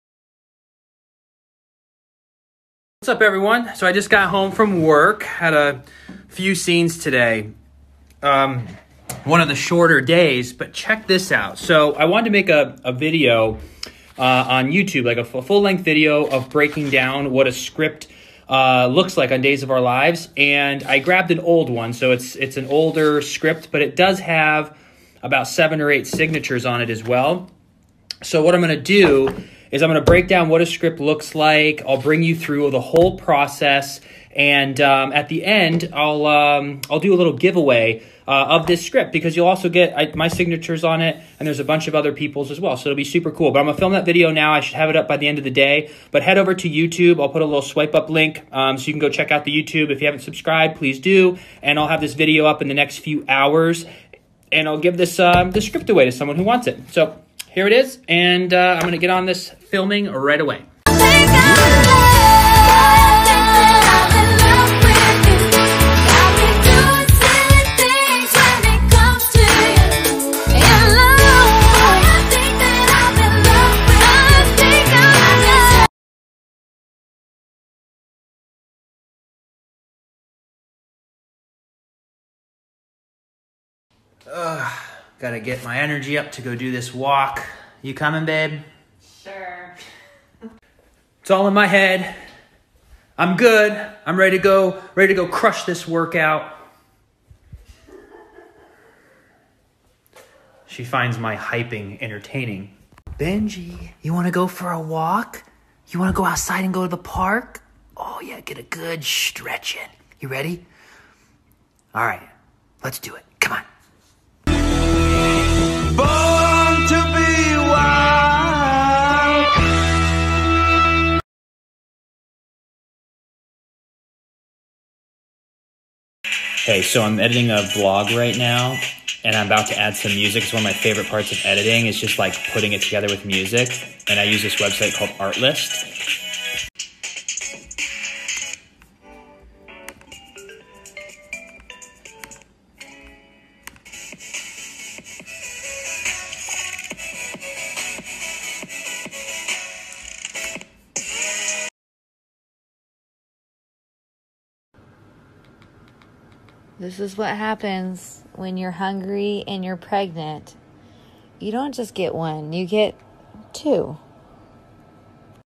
Freddie & Alyssa 2/6/20
A Day In The Life: Creating a video-breaking down a Days script, getting ready for a walk, Benji, editing a vlog